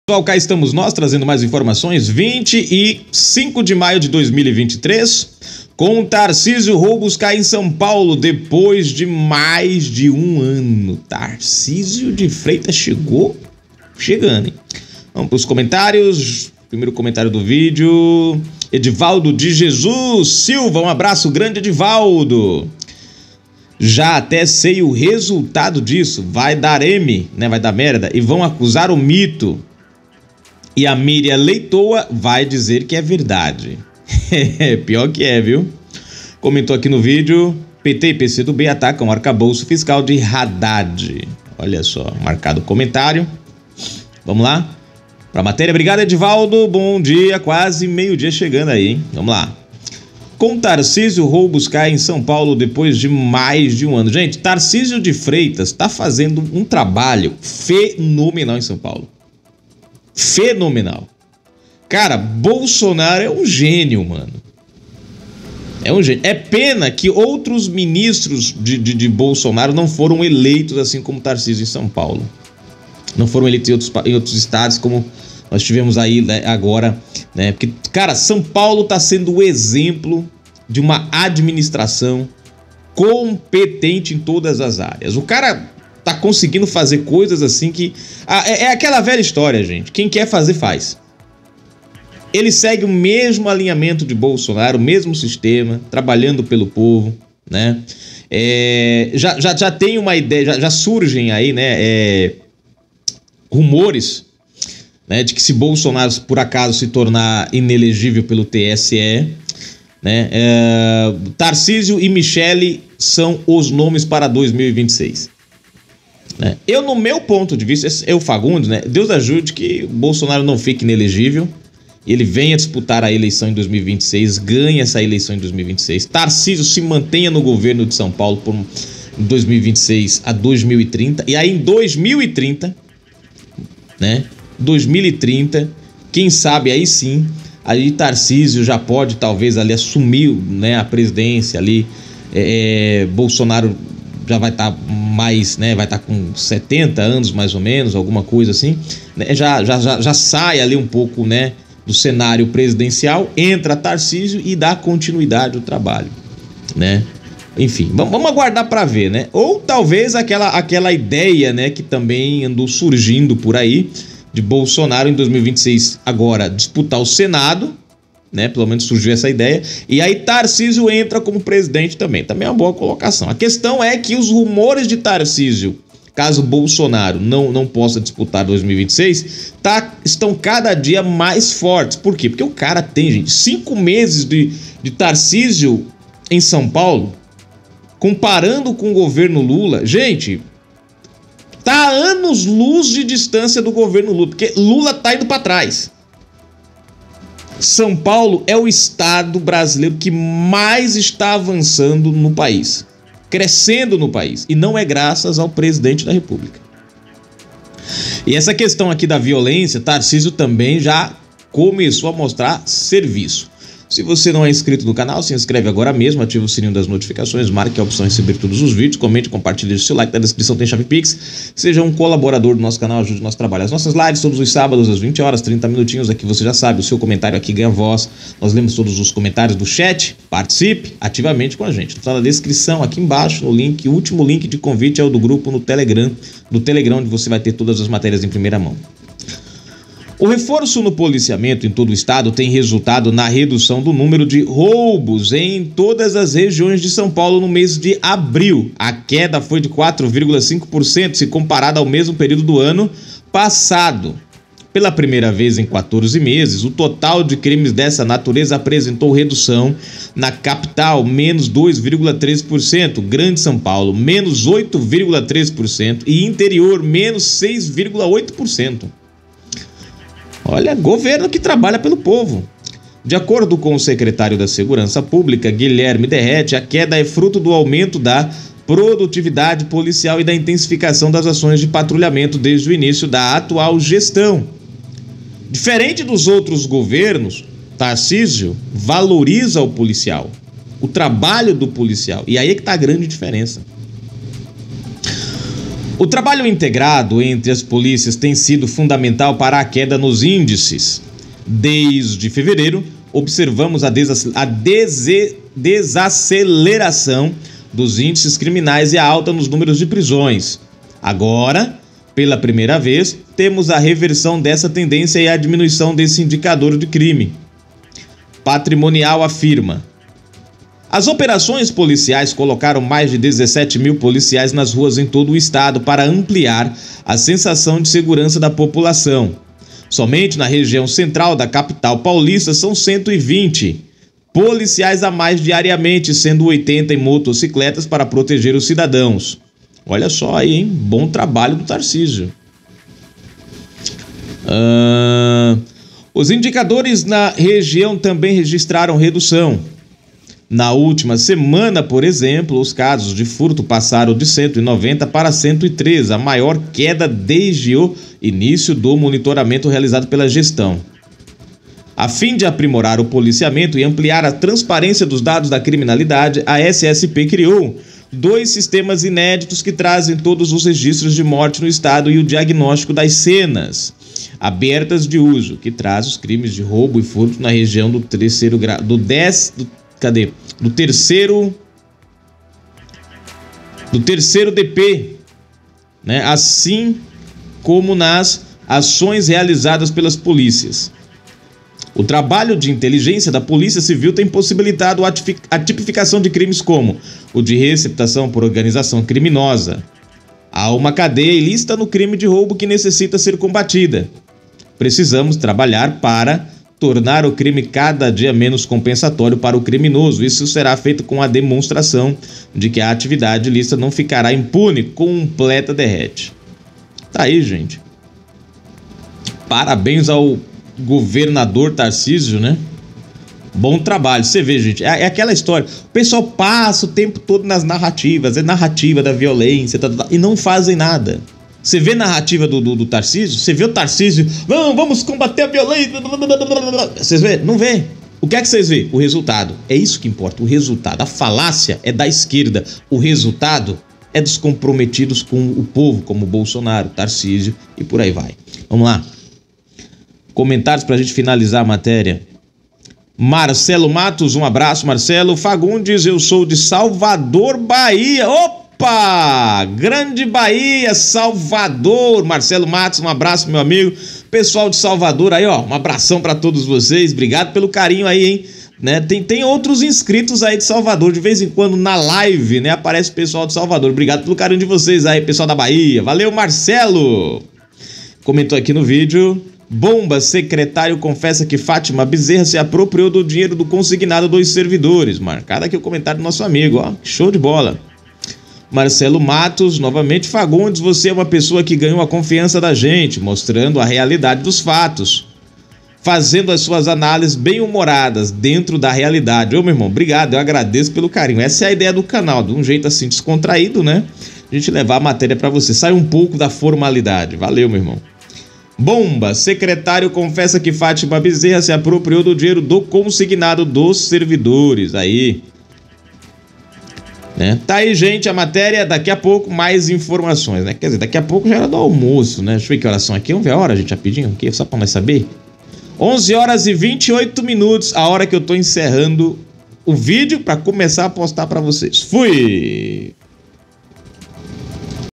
Após 5 meses no comando de SP, Tarcísio põe freio na criminalidade e índice de roubos cai no estado!
O reforço no policiamento em todo o estado tem resultado na redução do número de roubos em todas as regiões de São Paulo no mês de abril. A queda foi de 4,5% se comparada ao mesmo período do ano passado. Pela primeira vez em 14 meses, o total de crimes dessa natureza apresentou redução na Capital (-2,3%), Grande São Paulo (-8,3%) e Interior (-6,8%).

Apoiadores Maio:

Maria Antônia Siqueira Nunes.